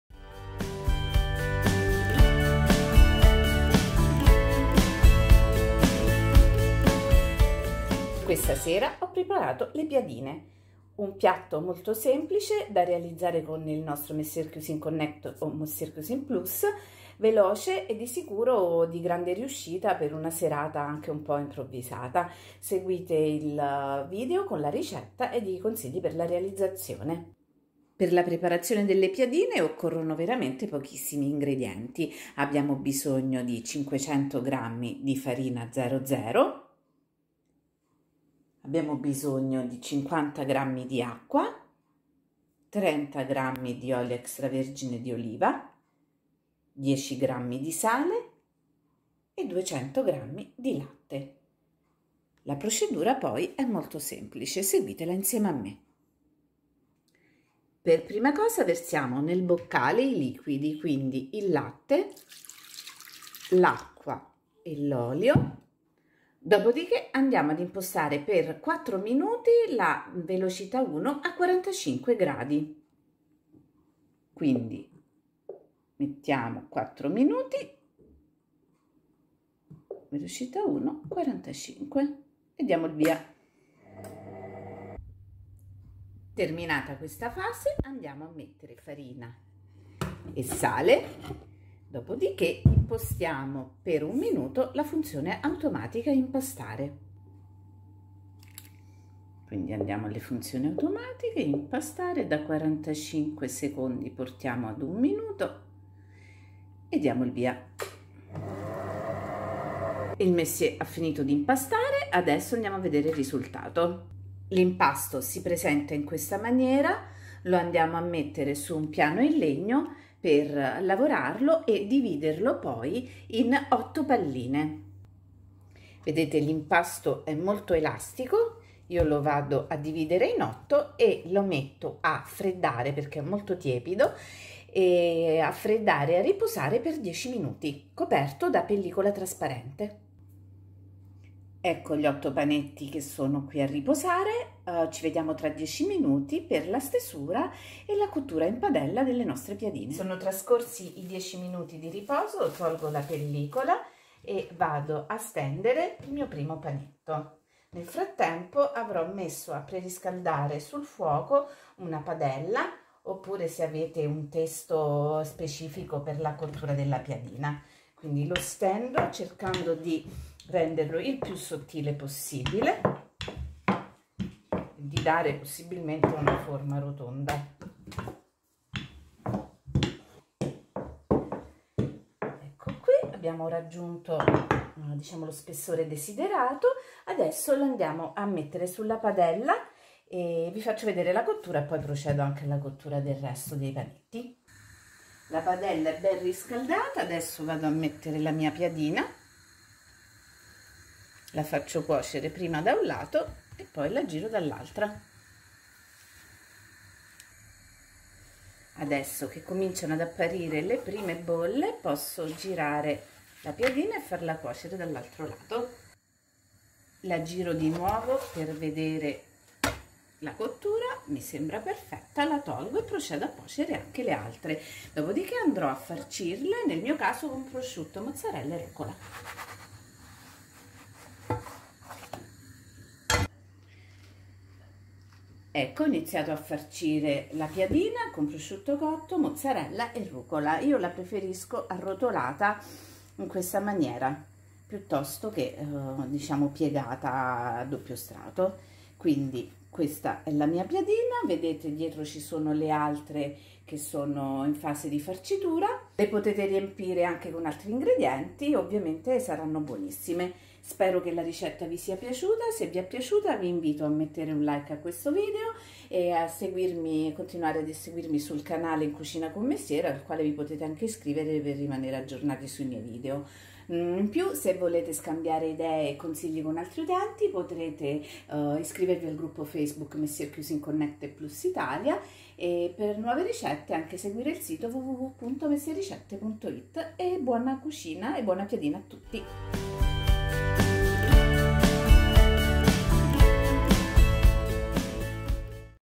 Piadine fatte in casa con Monsieur Cuisine Connect e Plus
La ricetta, gli ingredienti ed il procedimento delle Piadine fatte in casa con il Monsieur Cuisine Connect o con il Monsieur Cuisine Plus, tutte da farcire con i vostri gusti preferiti

CLICCA SULLA CAMPANELLA PER ESSERE AVVISATO QUANDO PUBBLICHIAMO UNA NUOVA RICETTA!